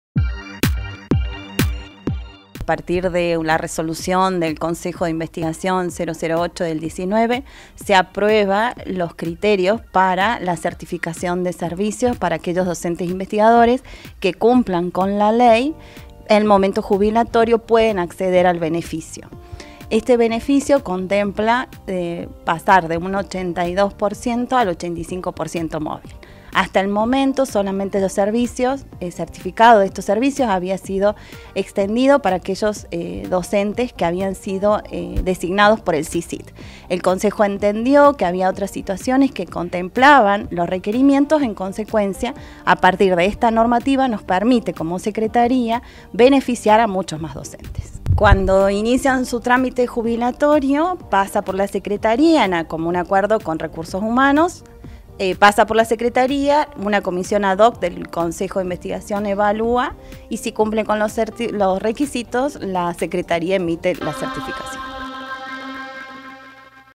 U Noticias Importantes cambios en el régimen previsional para docentes investigadores
Involucra un incremento a un 85% en el régimen previsional,  para docentes investigadores de la UNLaR